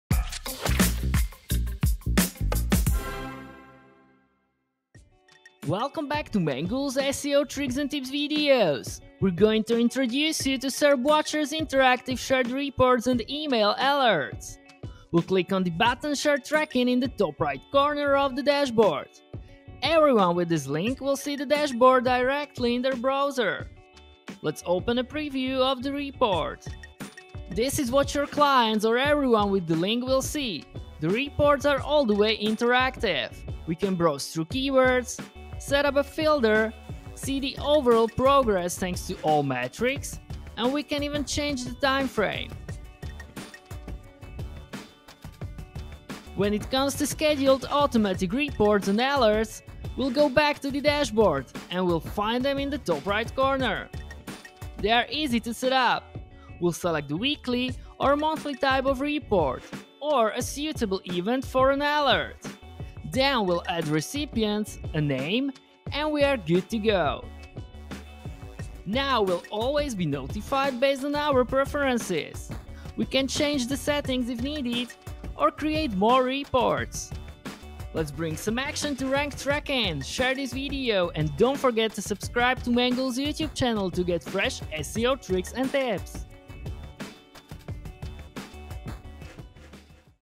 Interactive rank tracking reports & email alerts in SERPWatcher
Say hello to the SERPWatcher's interactive shared report! It's a rank tracking report with all metrics and preferred time frames directly in your browser.

Forget about out-of-date PDFs. It's all about being online and efficient.

We’ll click on the button Share tracking in the top right corner of the dashboard. Everyone with this link will see the dashboard directly in their browser.

Let’s open a preview of the report. It is what your clients or everyone with the link will see. The reports are all the way interactive.

When it comes to scheduled automatic reports and alerts, we’ll find them in the top right corner of the Dashboard. They are easy to set up.

SERPWatcher is developed by Mangools - Juicy SEO tools you will love ;)